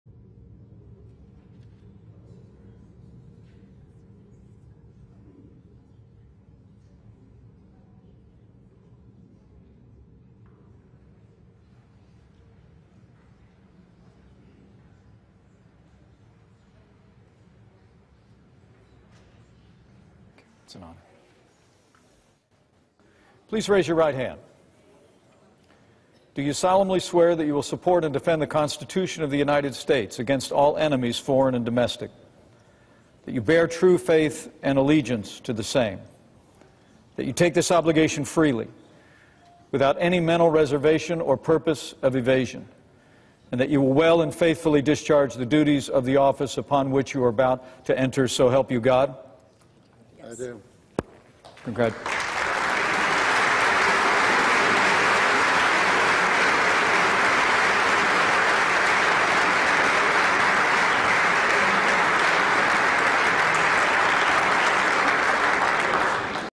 Elizabeth Warren, Others Sworn into New Senate
Pence swears in members of the new U.S. Senate class.